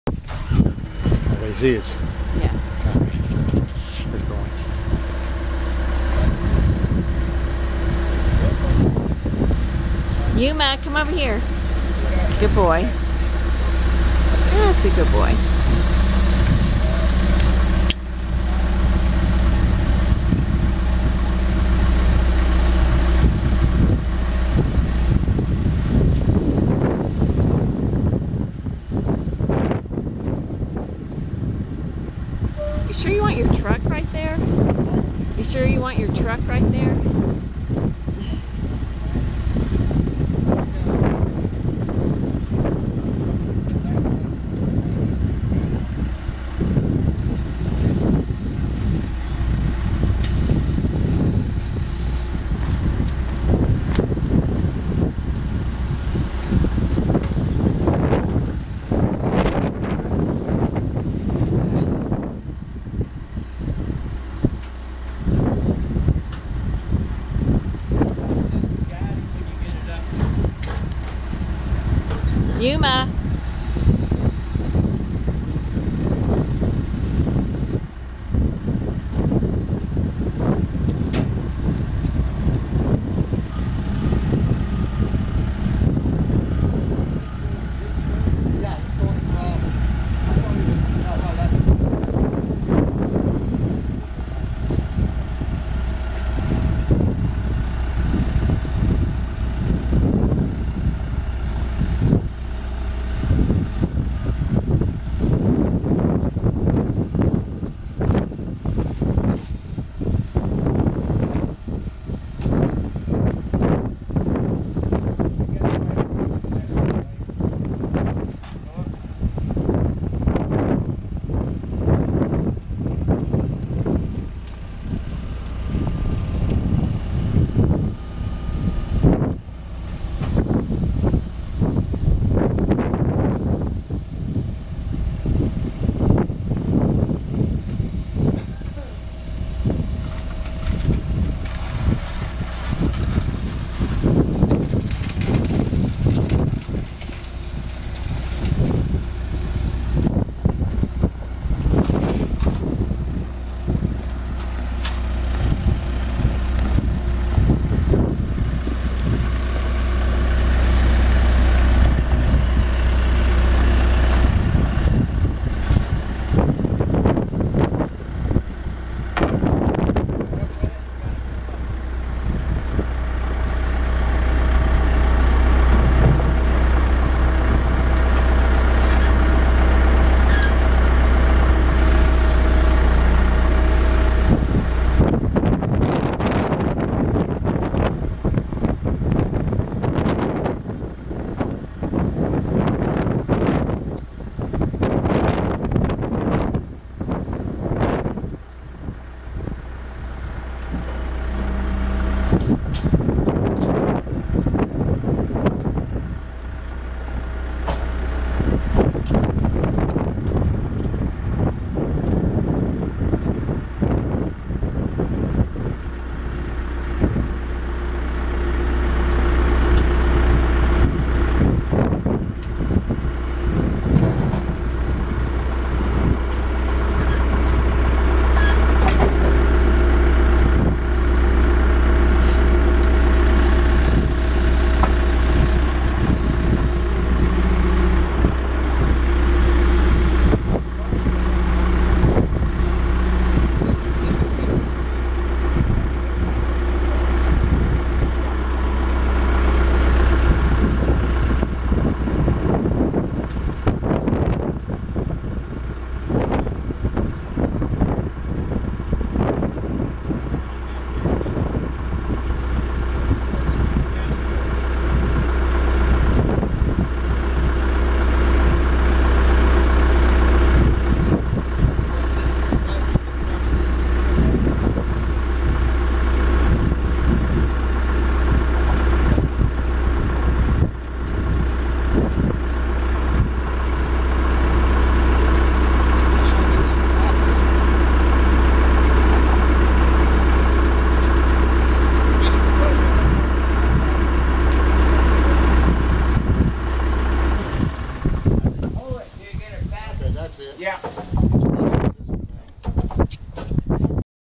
cross beam
Gordon using his backhoe to lift the 19 foot cross beam for the garage deck roof